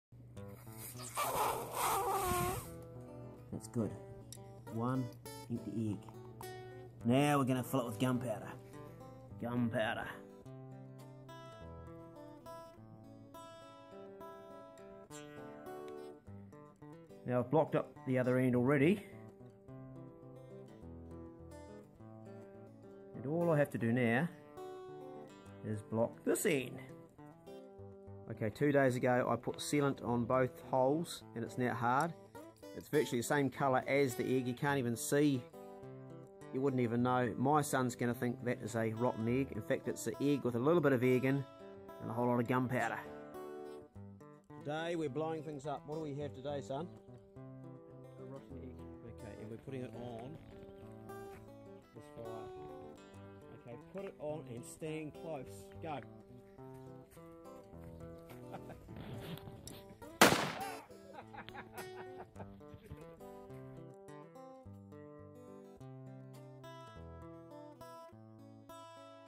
Eggsplosion
What happens if you replace the white & yoke inside an egg with gun powder, seal it and throw it into a raging fire?